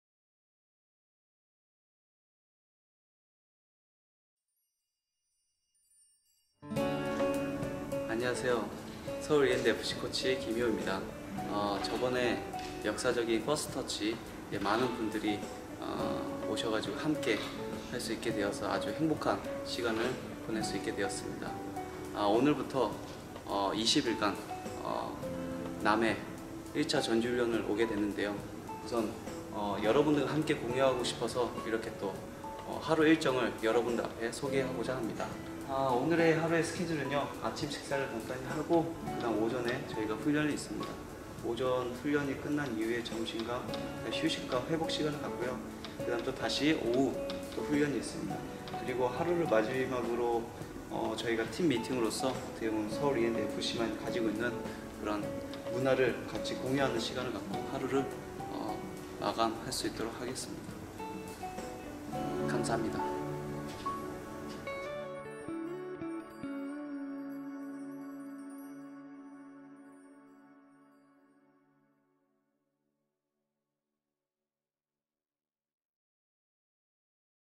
전훈 2일차 아침인사 - 김희호 코치, Preseason schedule briefing by Heeho Kim 2015.2.2
김희호 코치가 팬 여러분께 전하는 전지훈련 2일차 아침인사 및 스케줄 브리핑입니다.

Daily schedule briefing video of 2015 Preseason Tour by Heeho Kim (1st Team Coach)